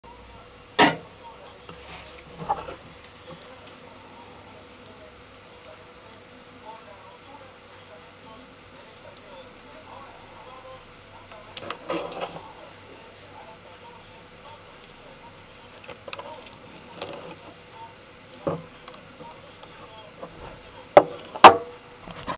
Cortando o Fileteando en Bicho del Caracol de Mar ya Cocido
Los tenes que hervor lo mínimo necesario para poder sacarlos.
El tiempo seria de de 5 a 10min no mas que eso.
Luego salen facil, y le cortas la parte de la tripa y el resto lo cortas en tiritas.
Luego las tiritas las preparas con aceite y ajo pimenton etc.
Y te armas como una conserva.
Se come frio.

El video lo filme en la cocina del depto, perdón por el desorden que había, pero la idea es mostrar que es posible cocinarlos y armar una linda, rica y sana picada.